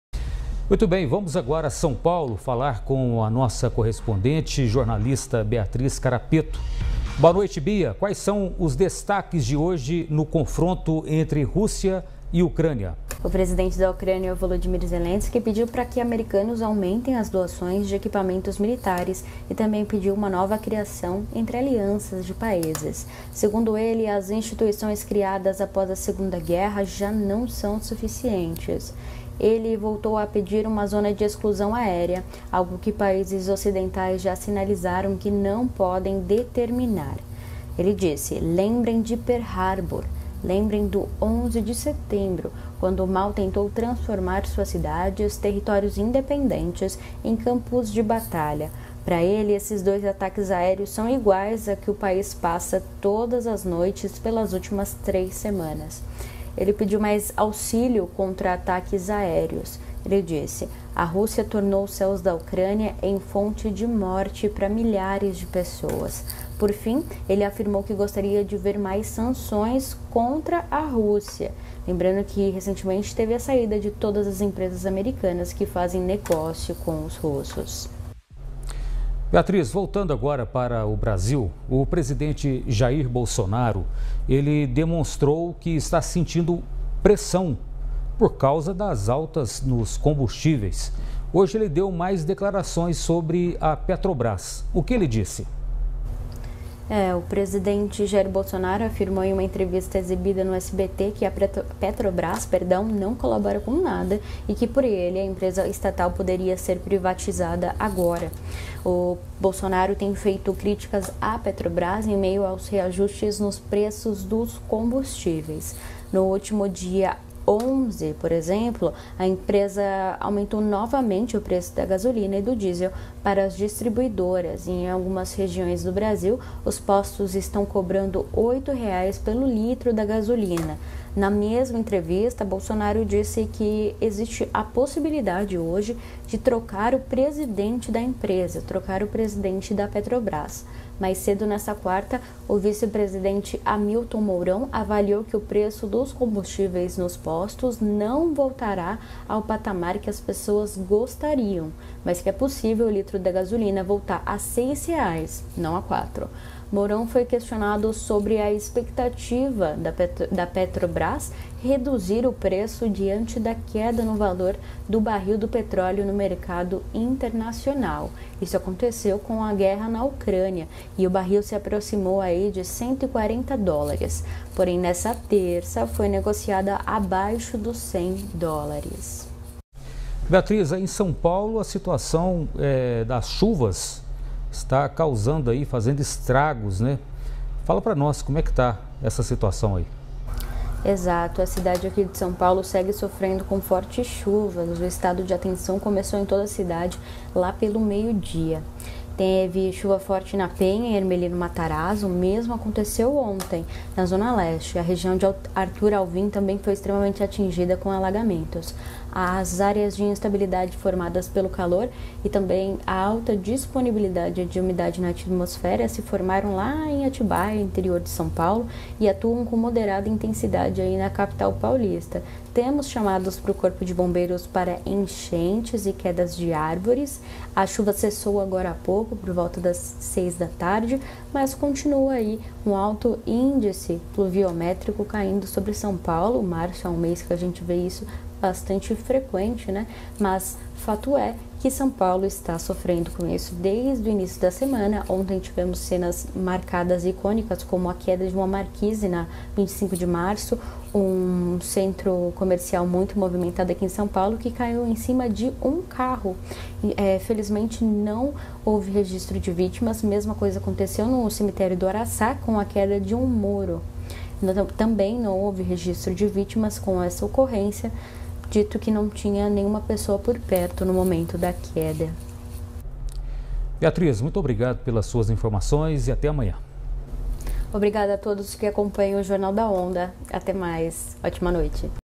CONFIRA COM BEATRIZ CARAPETO AS ATUALIZAÇÕES DE HOJE NA UCRÂNIA E AS INFORMAÇÕES AQUI NO BRASIL
| SÃO PAULO |